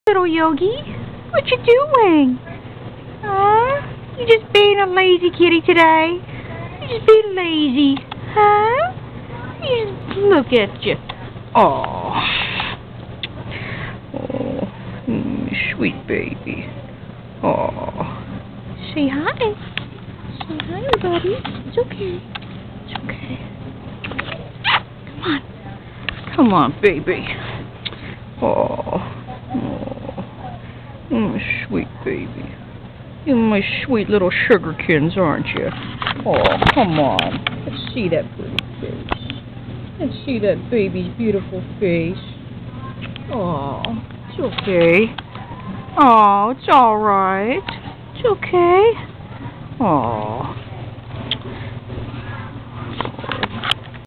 For my sweetest Kim (Therapist1972) With Lots of Love
My sweetest Kim, I hope Yogi will put a smile on your face with his cute, loveable kitty self.  We both love you lots!  We love the one & only, unique, special, sweet, precious, & beautiful you!  :)  Just another lazy afternoon with Yogi-Boo all stretched out on the carpet... Until he noticed he was being recorded.  :)  Please enjoy!